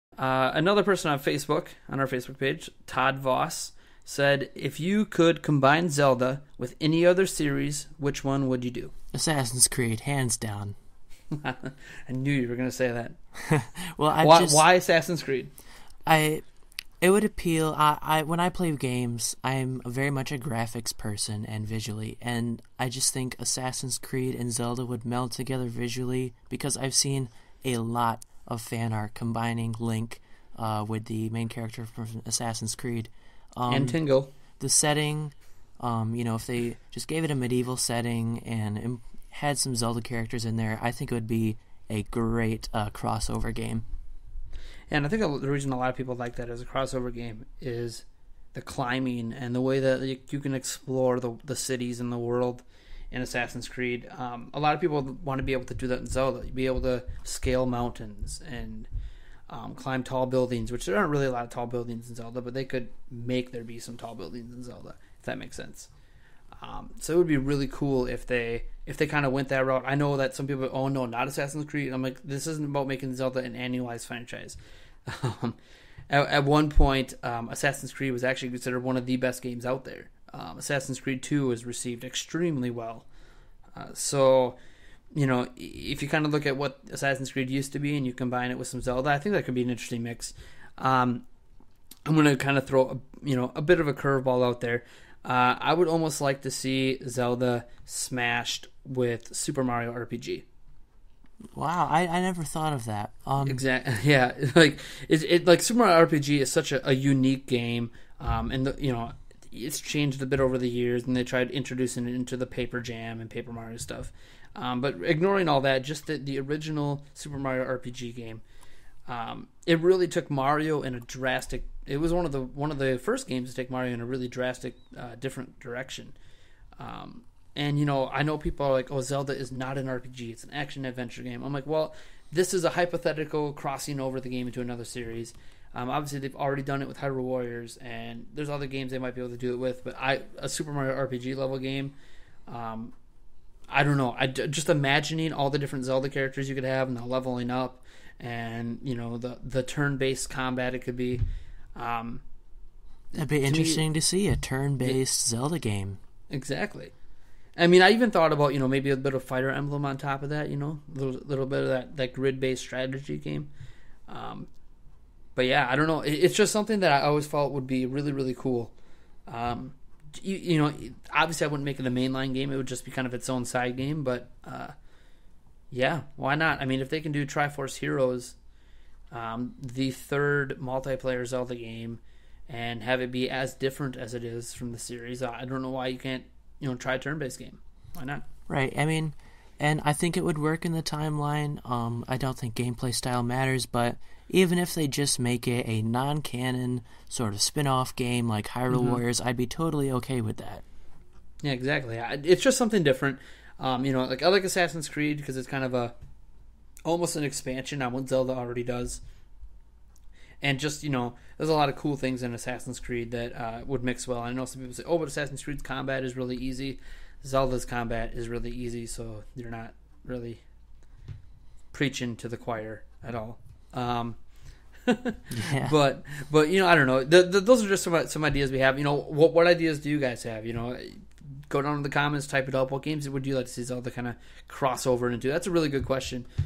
Zelda Inquiries: What's the Next Zelda Crossover Game?
We honestly don't know, but Darrin and Nate sit down to discuss our desires for future crossover Zelda titles.